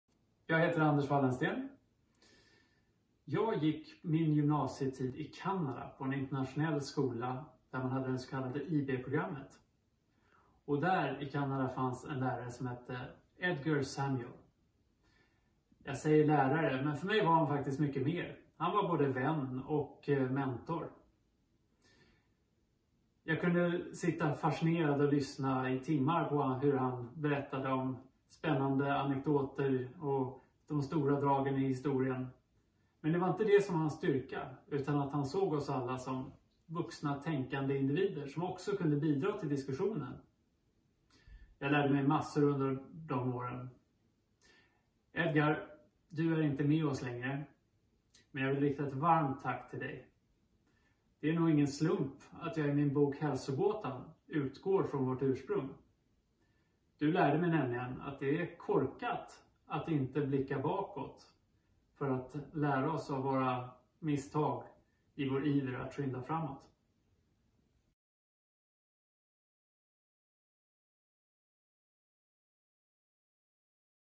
MIN LÄRARE: Anders Wallensten – ”Både vän och mentor”
Biträdande statsepidemiologen Anders Wallensten hyllar sin lärare från gymnasietiden i Kanada i videoserien "Min lärare".